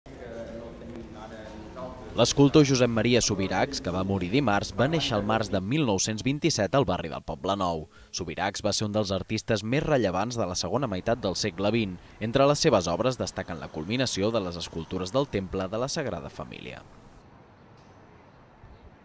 2014 04 10 Mort Subirachs
L’escultor Josep Maria Subirachs, que va morir dimarts va néixer el març de 1927 al barri del Poblenou. Subirachs va ser un dels artistes més destacats de la segona meitat del segle XX. Entre les seves obres destaquen la culminació de les escultures del temple de la Sagrada Família.